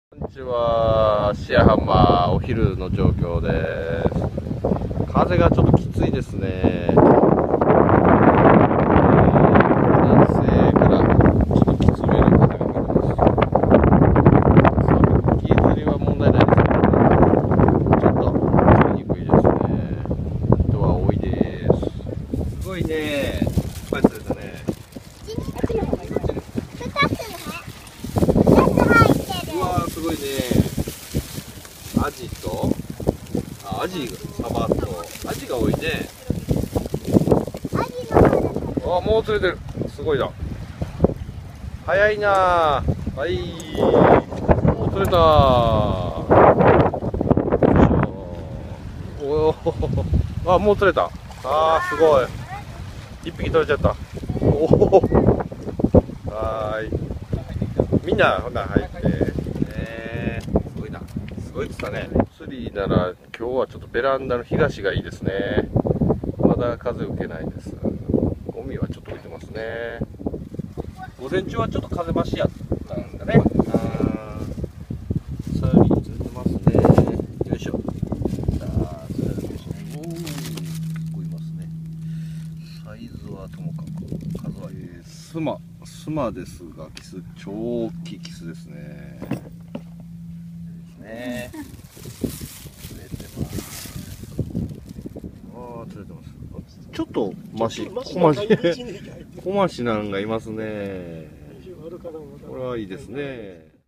fishingmax芦屋店 サビキ絶好調！サヨリも釣れます！
サビキ絶好調＼(^o^)／
サヨリちゃんも狙い目ですね( ´ ▽ ` )ﾉ